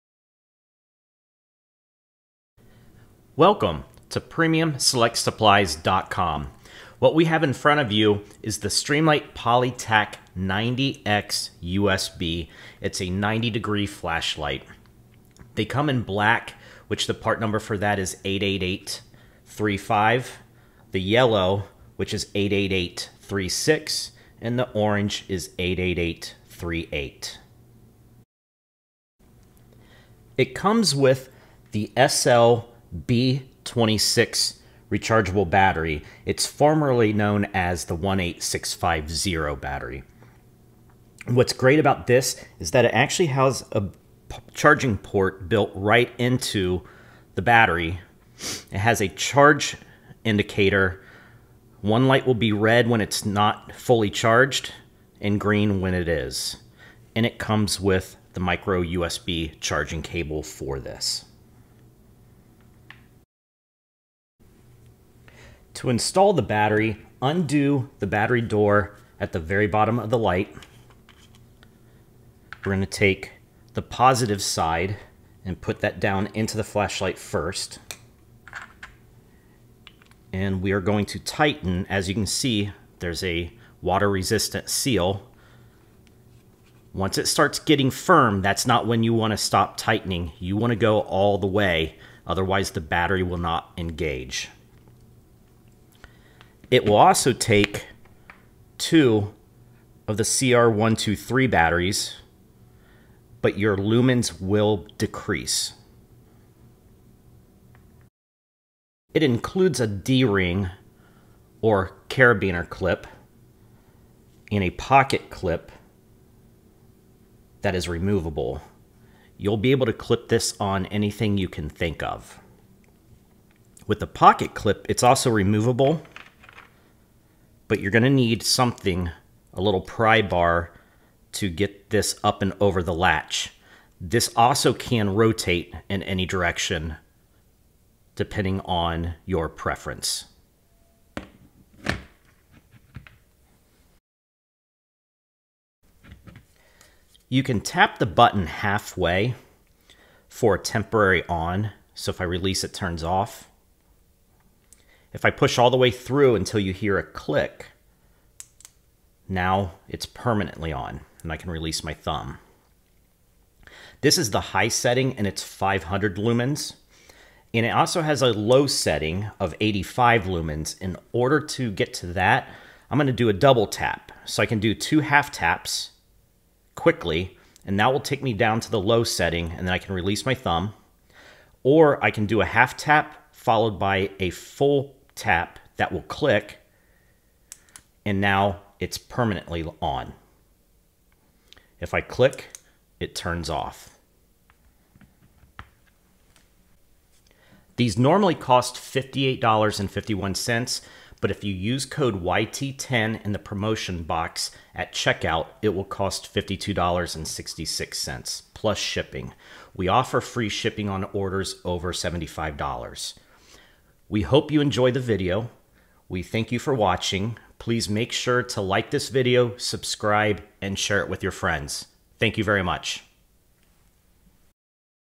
STREAMLIGHT POLYTAC 90 X USB 88835 88836 88838
Streamlight PolyTac 90X USB / PolyTac 90X Flashlight
Our Price: $58.51
Discount Price: $52.66 after code YT10 @ checkout

This compact tactical flashlight features an integrated carabiner style D-ring which allows for a variety of attachment methods. The light has an output of 500 lumens on high and a runtime of 19 hours on low. The on/off switch is a head-mounted push button which is easy to use with gloves. It can be used with either one Streamlight SL-B26 battery pack or two CR123A batteries.

Streamlight PolyTac 90X LED Flashlight Features
Multi-Fuel: Uses two (2) CR123A lithium batteries OR one Streamlight SL-B26 protected Li-Ion USB Rechargeable Battery Pack
Output:
High: 500 lumens
Low: 85 lumens
Beam Distance:
High: 167 m
Low: 64 m
Runtimes:
High: 2.5 hours (CR123A); 3.75 hours (SL-B26 battery pack)
Low: 12 hours (CR123A); 19 hours (SL-B26 battery pack)
2 meter impact resistance tested
IPX7 rated design - waterproof to one meter for 30 minutes
Multi-function push button head switch is easy to use with gloves. Easily change modes.
Fully rotating/reversible / removable clip
Optional Gear Keeper features a retractable attachment system
Non-Rechargeable
Length: 4.43"
Head Diameter: 1.30"
Body Diameter: 1.11"
Weight: 4.8 oz (with SL-B25 rechargeable battery), 4.3 oz (with CR123A batteries)
Color: Black,  Yellow, Orange
Streamlight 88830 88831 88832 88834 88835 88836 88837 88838

Streamlight PolyTac 90X Flashlight has a Limited Lifetime Warranty against defects.

Authorized Streamlight Dealer